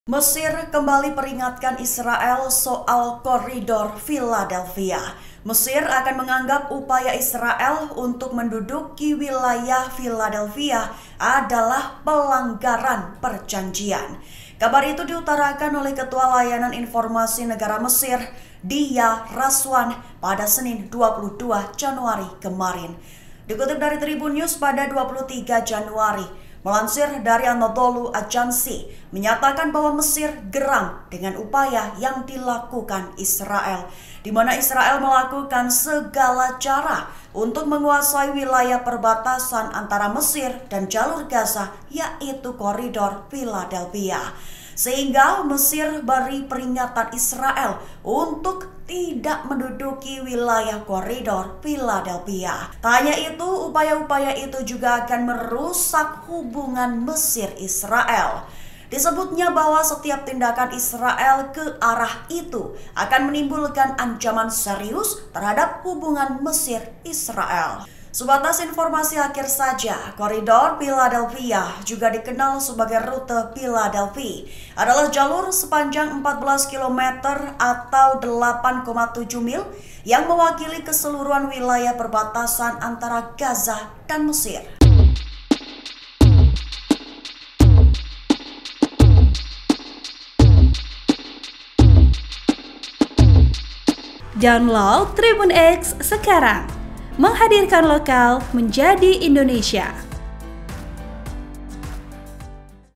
Mesir Peringatkan Israel soal Koridor Philadelphia: Jangan Langgar Perjanjian dan Protokol Keamanan
TRIBUN-VIDEO.COM - Mesir kembali peringatkan Israel soal koridor Philadelphia.

Mesir akan menganggap upaya Israel untuk menduduki wilayah Philadelphia adalah pelanggaran perjanjian.

Kabar itu diutarakan oleh Ketua Layanan Informasi Negara Mesir (SIS), Diaa Rashwan pada Senin (22/1/2024).

Dikutip dari Tribunnews pada (23/1) melansir dari Anadolu Ajansi, menyatakan bahwa Mesir geram dengan upaya yang dilakukan Israel.

Di mana, Israel melakukan segala cara untuk menguasai wilayah perbatasan antara Mesir dan Jalur Gaza yaitu koridor Philadeplhia.

Sehingga, Mesir beri peringatan Israel untuk 'tidak menduduki wilayah koridor Philadelphia'.

"Ini pelanggaran terhadap perjanjian dan protokol keamanan yang ditandatangani antara Israel dan Mesir," katanya, dikutip dari Anadolu Ajansi.

Tak hanya itu, upaya-upaya itu juga akan merusak hubungan Mesir-Israel.

Disebutnya bahwa, setiap tindakan Israel ke arah 'itu' akan menimbulkan ancaman serius terhadap hubungan Mesir-Israel.

“Harus ditekankan dengan tegas bahwa setiap tindakan Israel ke arah ini akan menimbulkan ancaman serius terhadap hubungan Mesir-Israel,” tambahnya.

Sebatas informasi akhir saja, koridor Philadelphi, juga dikenal sebagai Rute Philadelphi, adalah jalur sepanjang 14 km (8,7 mil) yang mewakili keseluruhan wilayah perbatasan antara Gaza dan Mesir.

(Tribun-Video.com)

Artikel ini telah tayang di Tribunnews.com dengan judul Israel Bertekad Kuasai Koridor Philadelphia, Mesir: Ini Melanggar Perjanjian,
Penulis: Farrah Putri Affifah
Editor: Bobby Wiratama